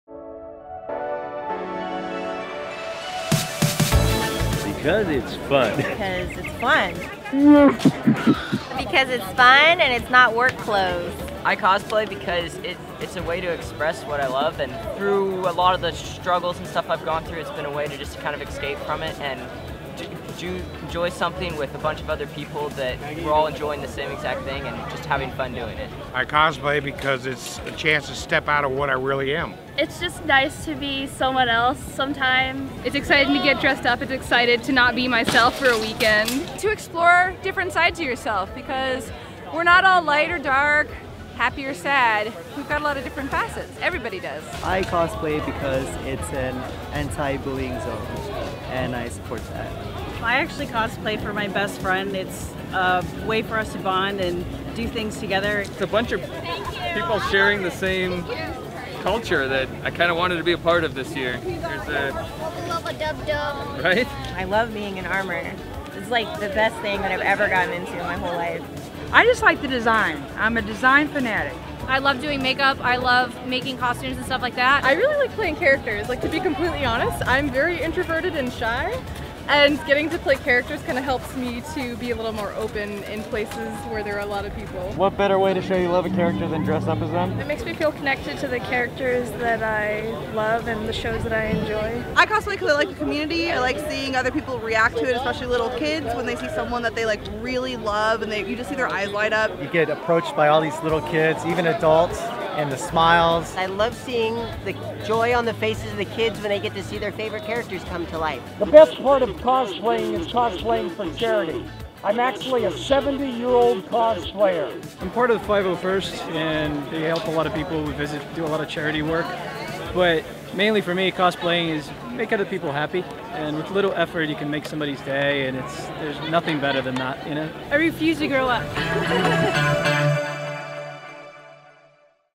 Why Do You Cosplay?: Comic-Con | Los Angeles Times
Elaborate costumes? Rivers of back sweat. Hard to eat and drink. Why would anyone cosplay? Turns out, there are many reasons to put on your favorite costume and go to Comic-Con.